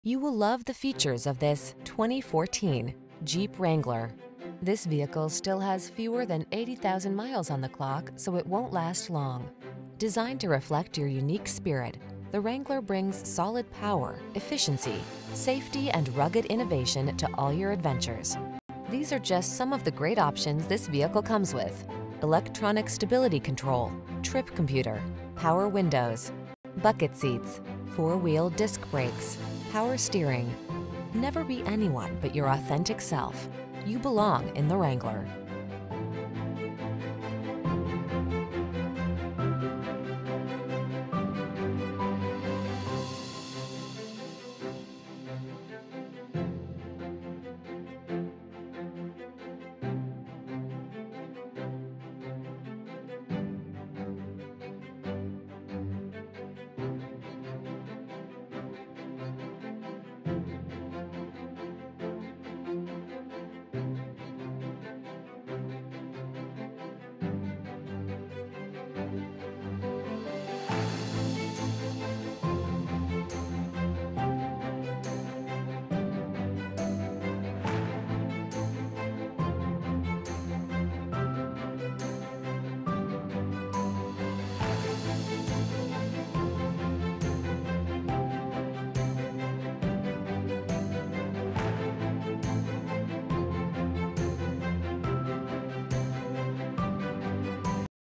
2014 Jeep Wrangler New Bern NC, Jacksonville NC, Wilson NC, Greenville NC, Goldsboro P4997
Flame Red Clearcoat Used 2014 Jeep Wrangler available in Greenville, North Carolina at Brown and Wood Cadillac GMC Mazda. Servicing the New Bern, Jacksonville, Wilson, Greenville, Goldsboro, NC area.

329 SW Greenville Blvd.